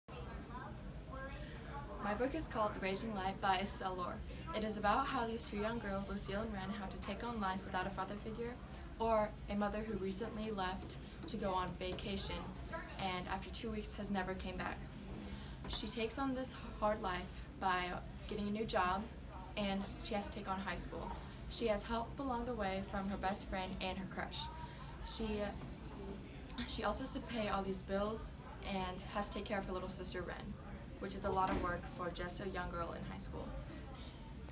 This Raging Light by Estelle Laure
Student book review over the YA novel This Raging Light by Estelle Laure